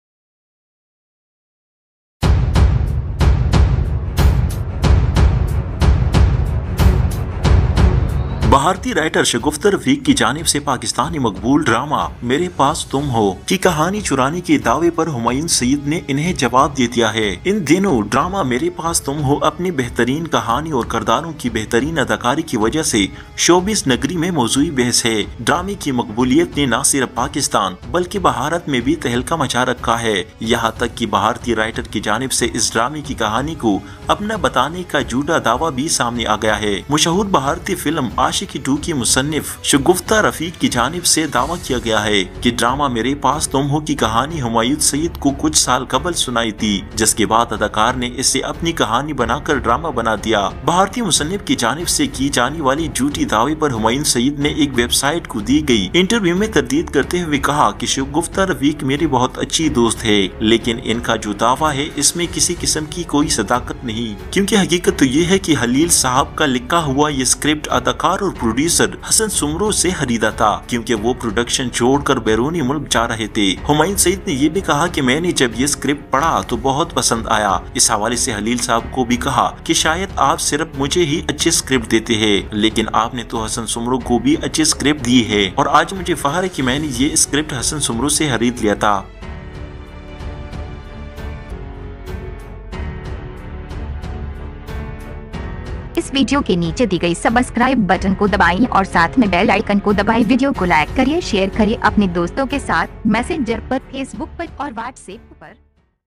Meray Paas Tum Ho ||Bollywood allegations Humayun saeed
Meray Paas Tum Ho || Humayun Saeed respond to Indian Bollywood allegations

Indian Bollywood writter shagufta rafique Said that Humayun saeed stolen my story and made  Mere Pass tum ho Drama which is now on air on Ary Digital .

Mere Pass Tum ho Episode 15 already on air on Ary digital 30th November 2019
and Remaining episode 16 , 17  will be on air on Upcoming Days  .